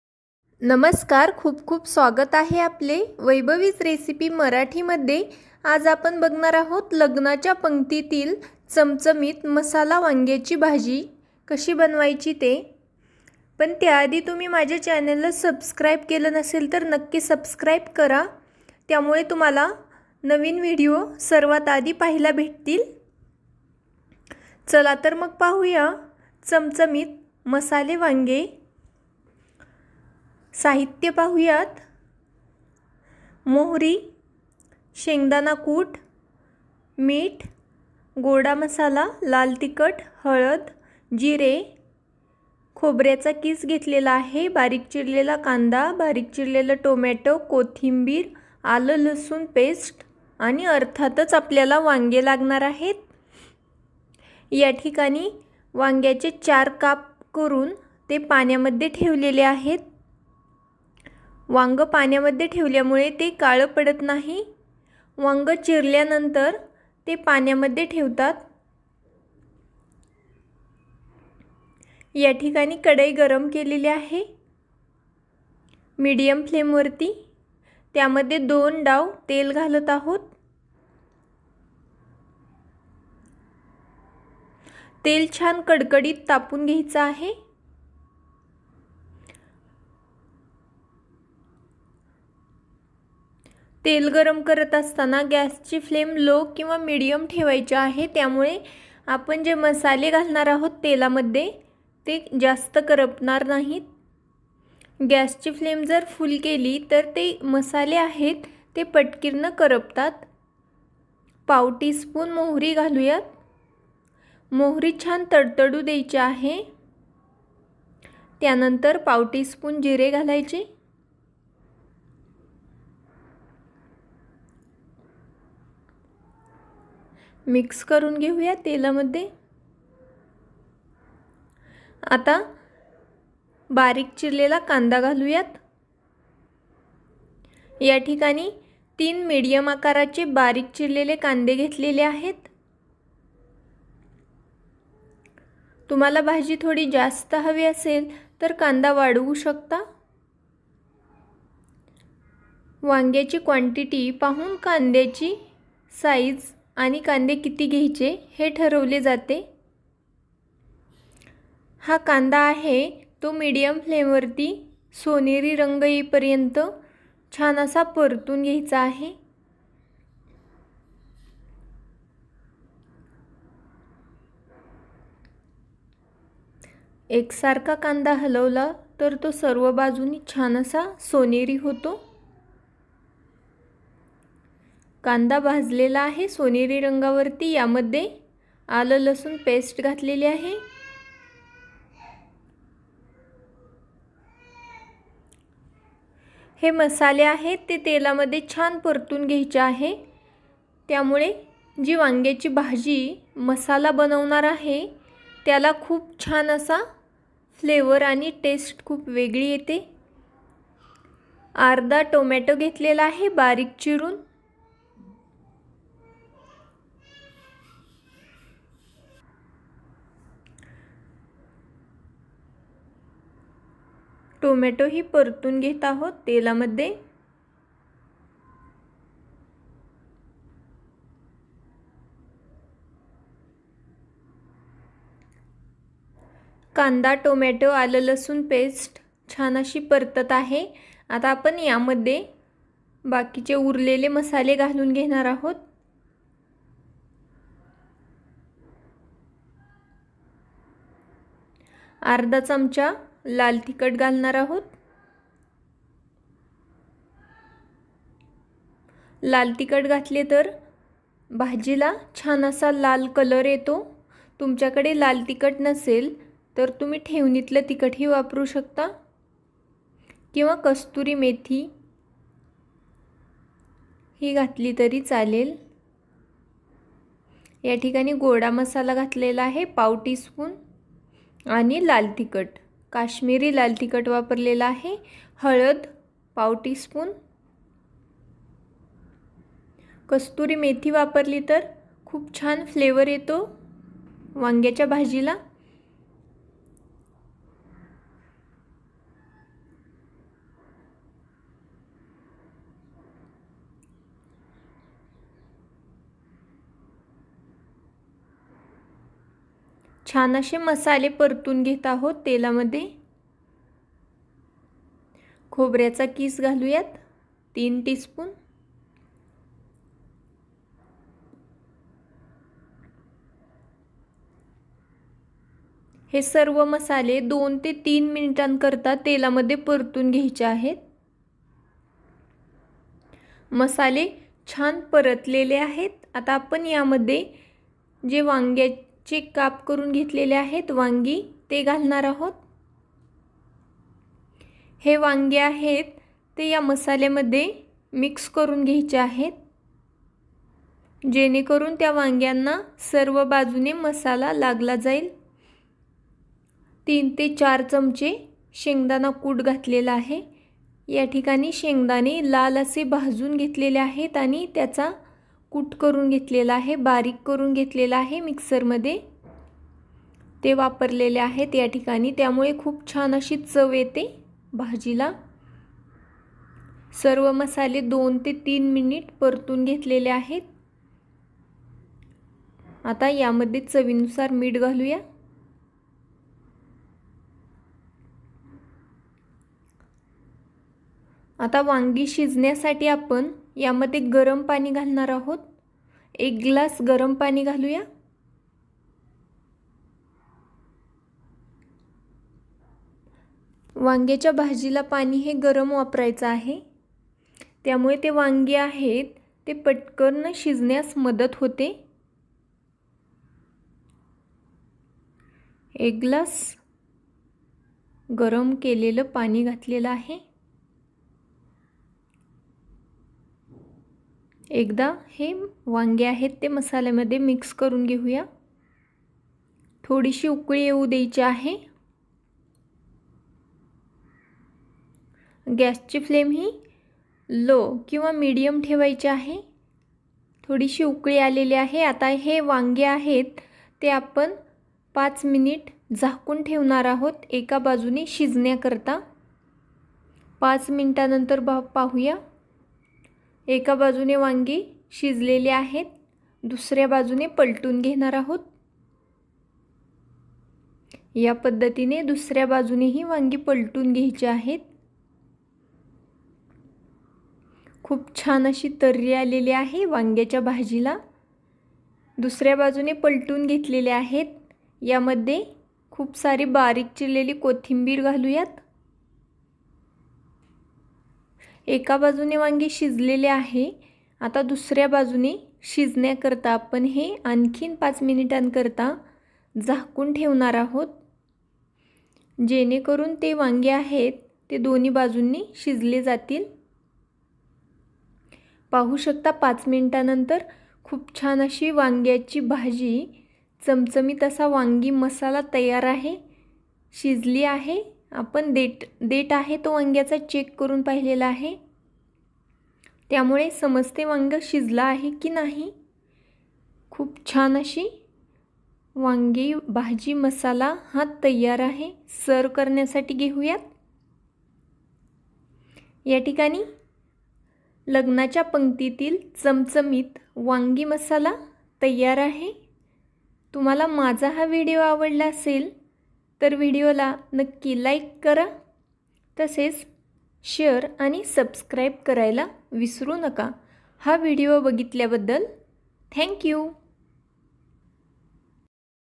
मसाला वांगी | लग्नाच्या पंगतीतील चमचमीत मसाला वांगी | masala vangi recipe by vaibhavisrecipe
साहित्य:
वांगी
कांदा
टोमॅटो
आल लसुण पेस्ट
कोथिंबीर
जिरे
मोहरी
गोडा मसाला
हळद
मीठ
लाल तिखट
शेंगदाणा कूट